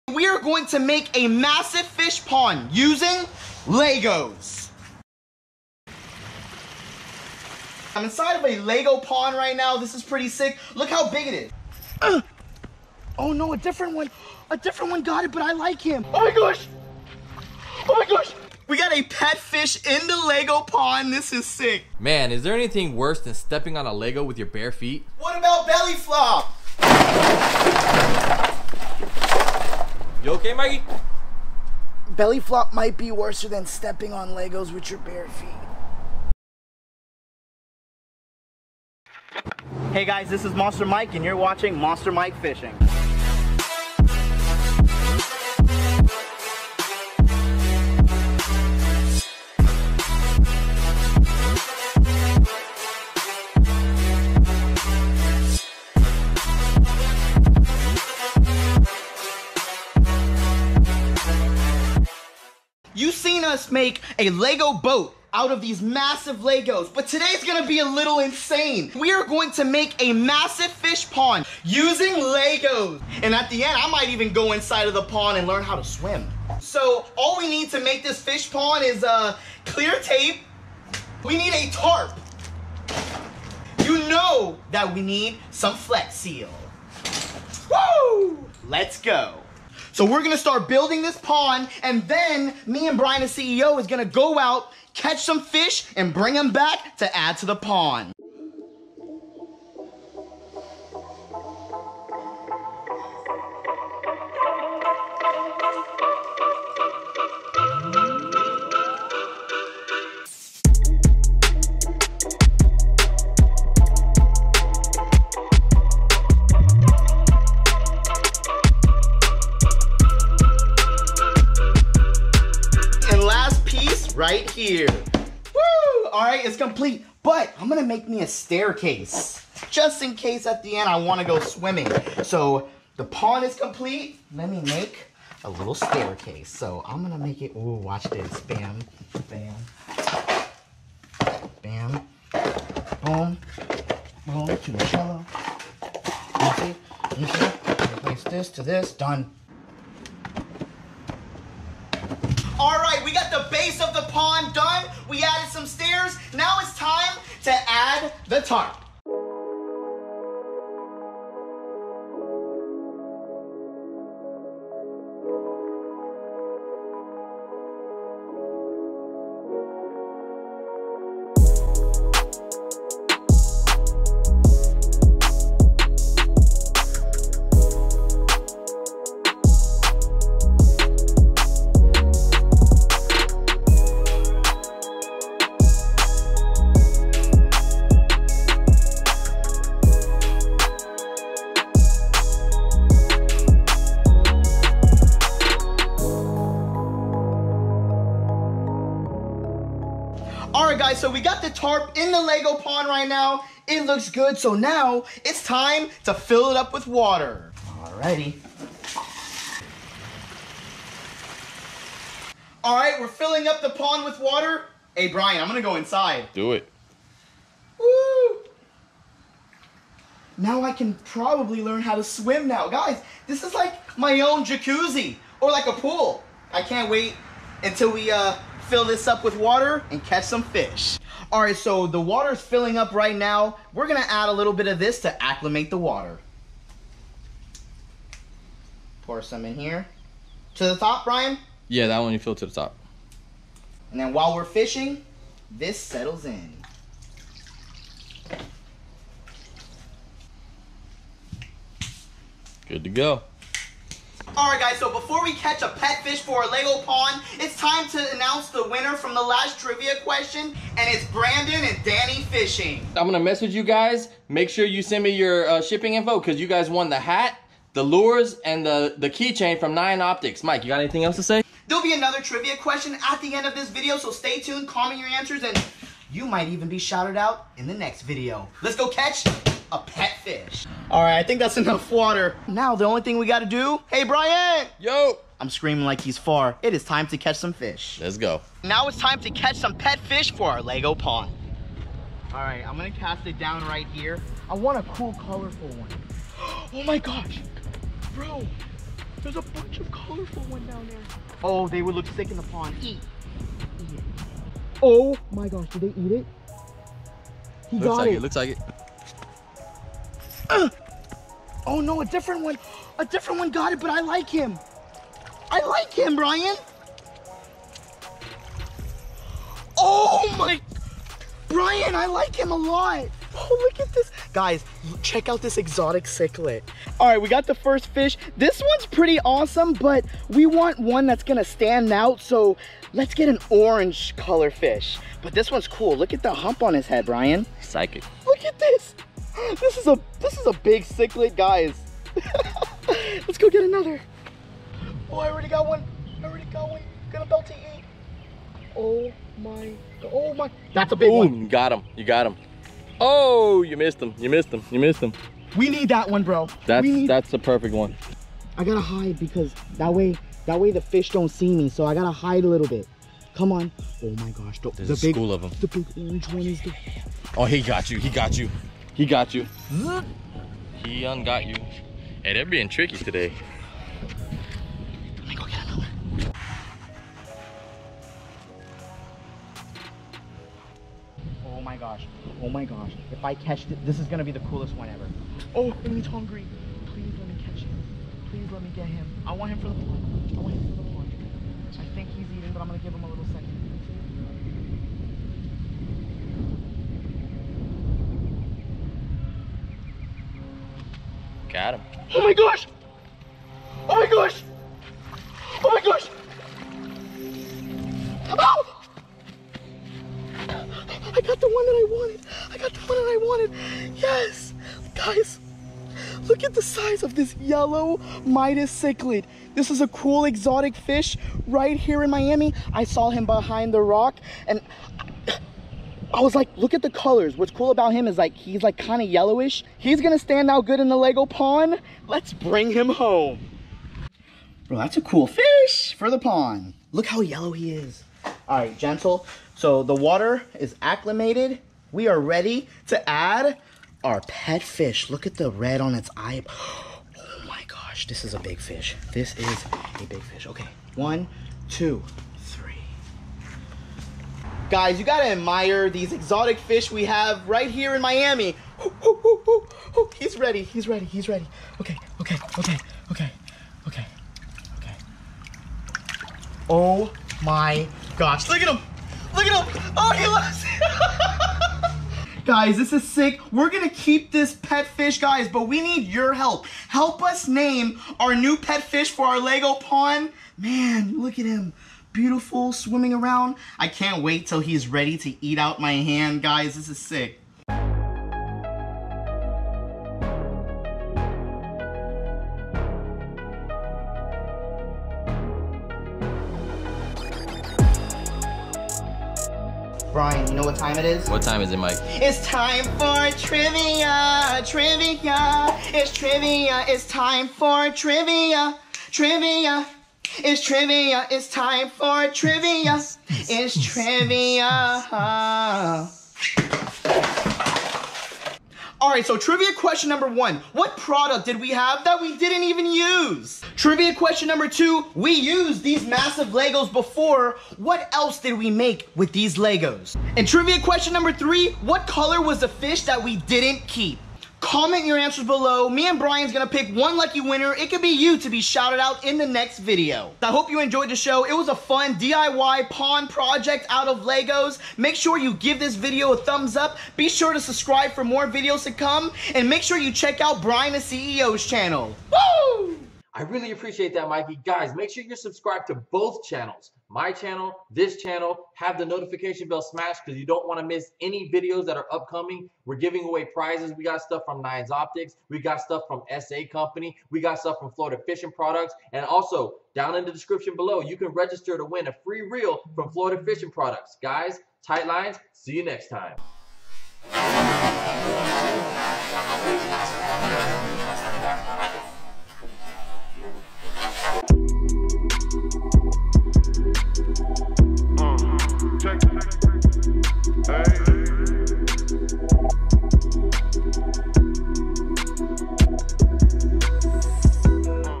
NEW Giant LEGO Fish POND!!! DIY
How to: DIY LEGO Fish Pond for exotic fish!!! We show you how to make it and take you fishing with us to catch our new pet fish!